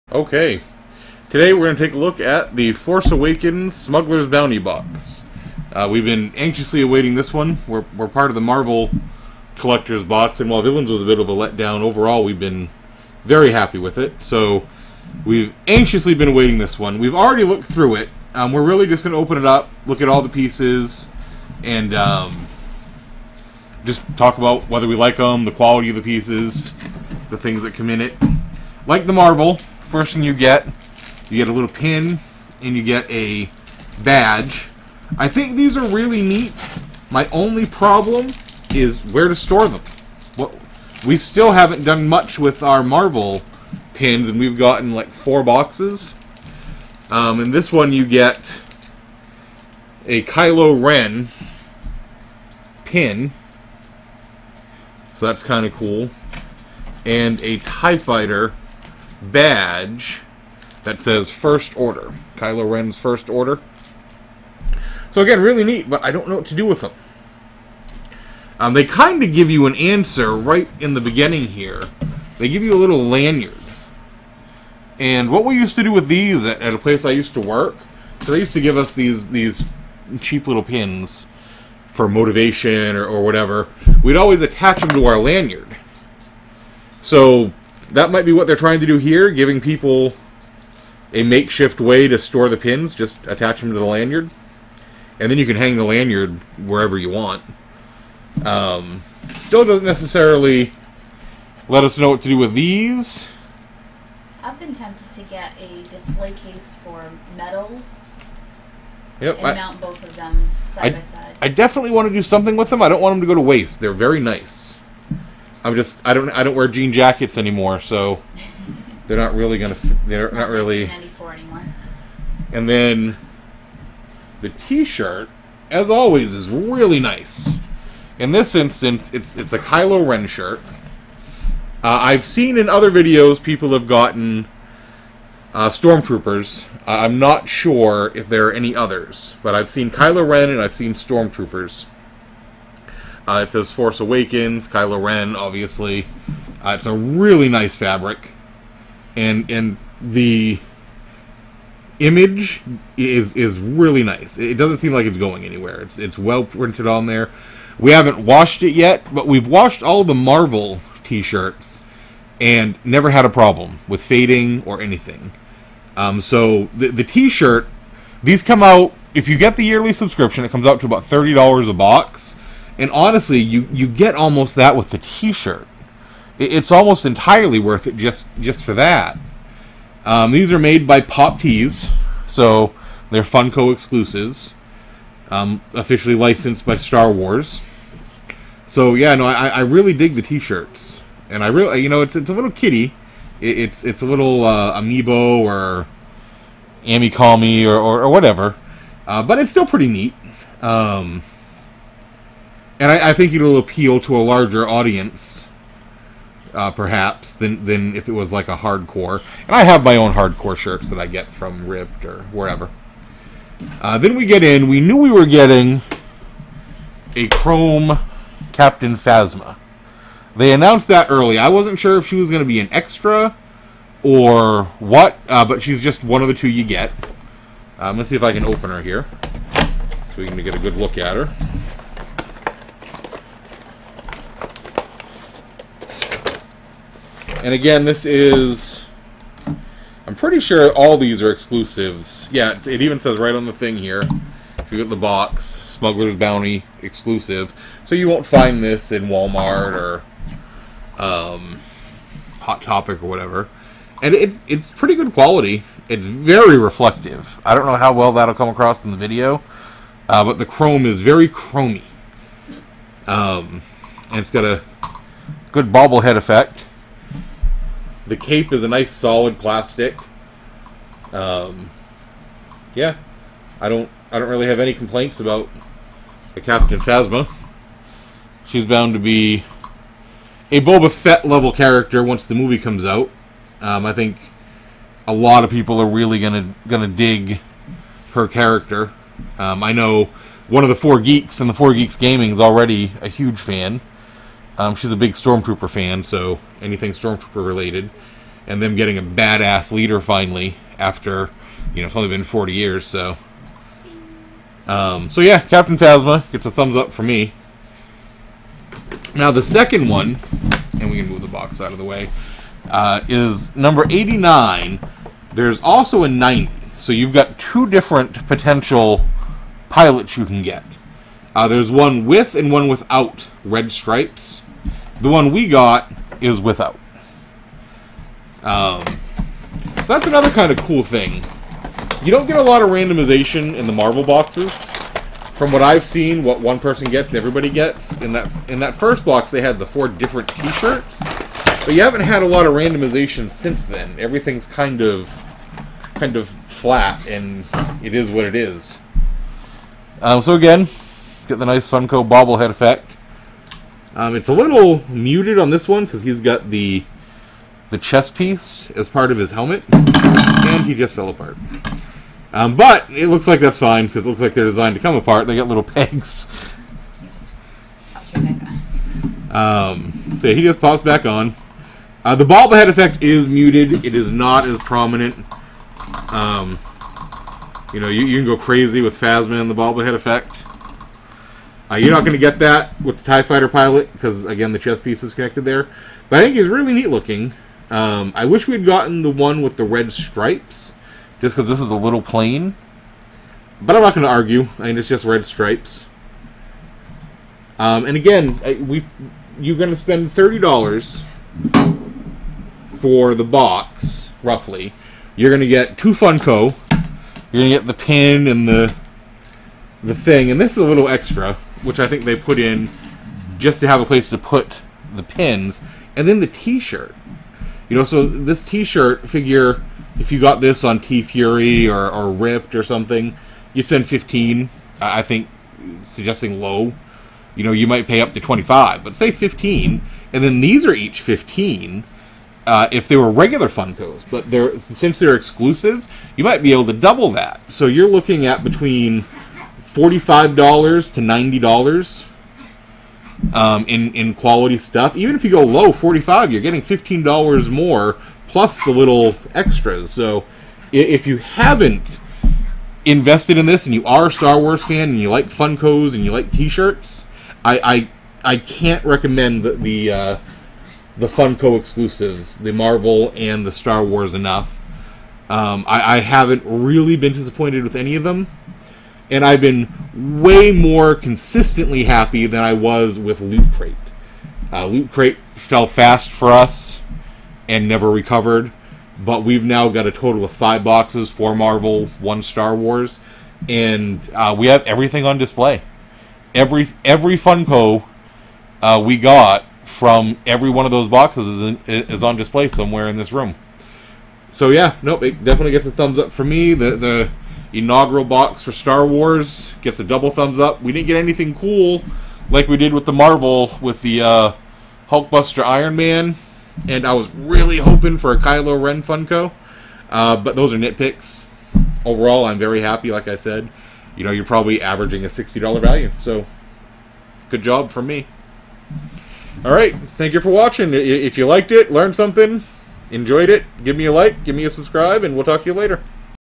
Smuggler's Bounty November 2015 Review
We review the November box for Funko's Smuggler's Bounty.